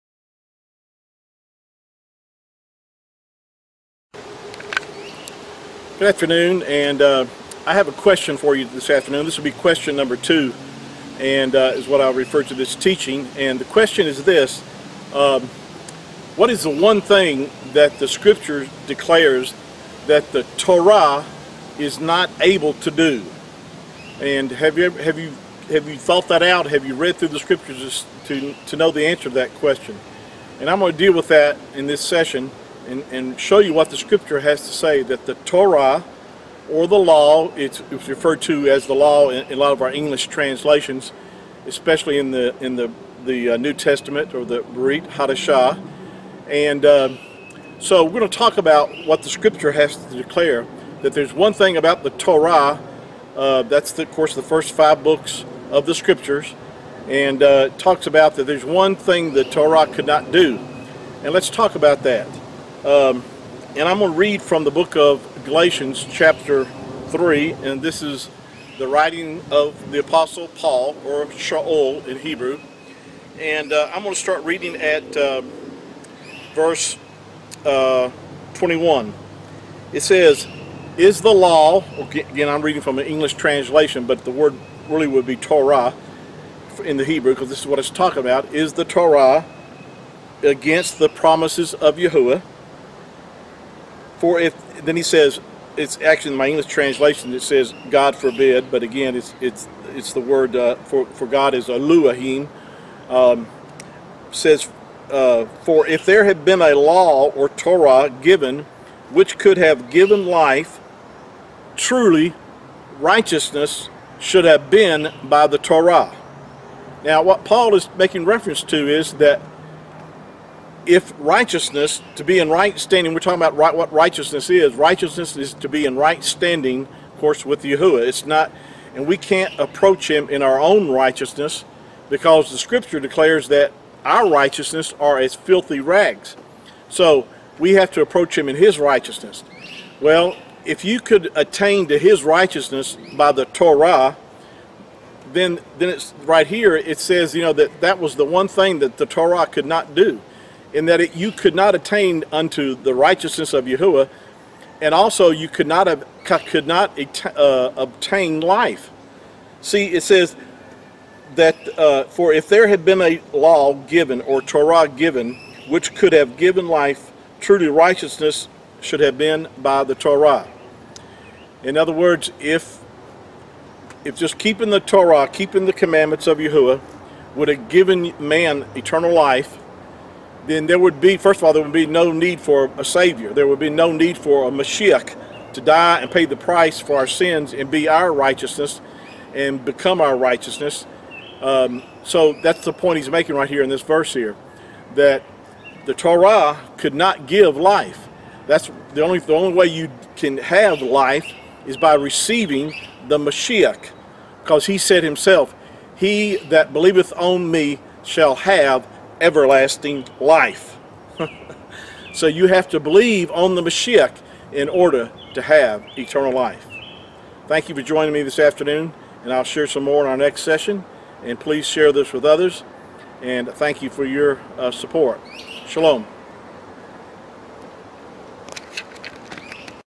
Yahuah said thru the Apostle Paul that the Torah could not give life!
Eternal Life is found in Yahusha!